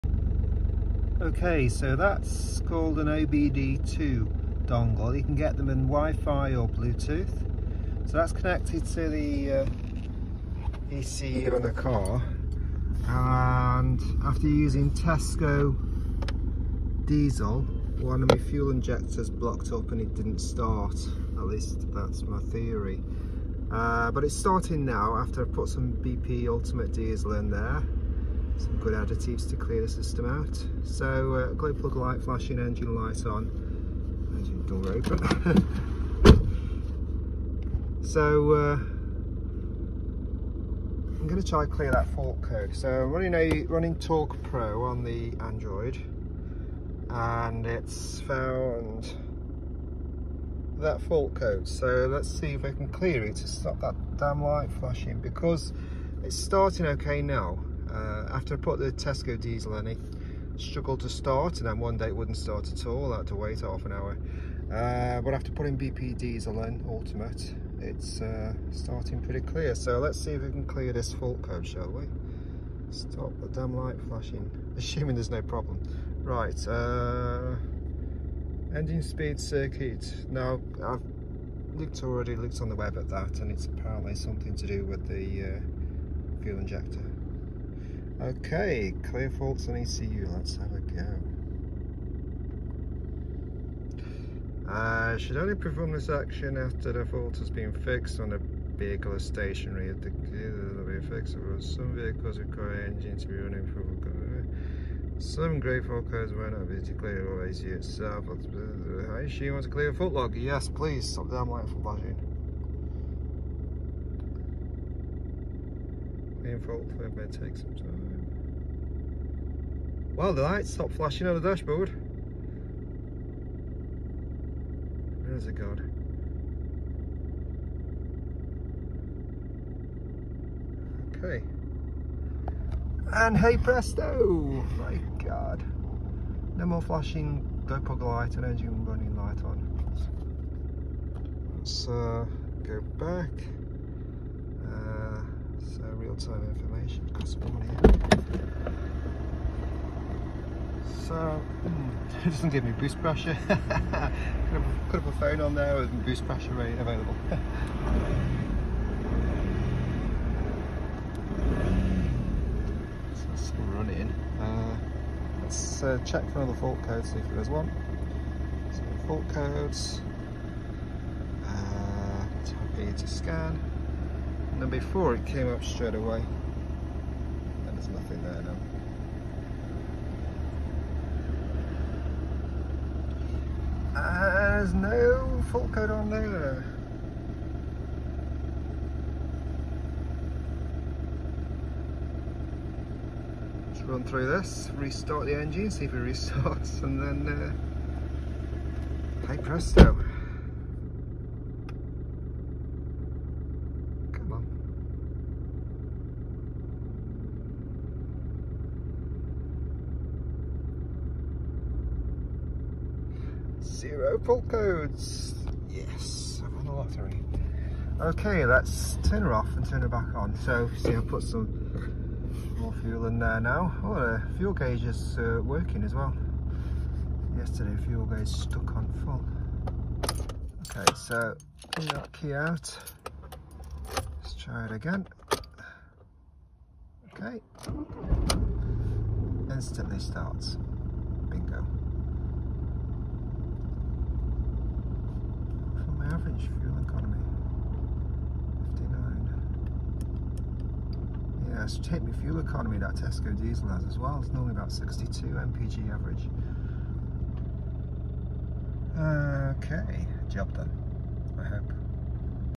Clear fault code after using cheap diesel
My mechanic said don’t use Tesco sh** diesel as it has no additives, but I gave it a shot as it’s a pound a gallon cheaper. After a few miles the Audi A2 1.4 TDI 3cyl started running slower with a hotter than normal engine temp. Also, it was taking longer to start the engine until 1 day it would not start. Used an OBD2 dongle to clear fault code & still ok 2weeks later with more expensive diesel… plus driving noticeably faster & now starting instantly.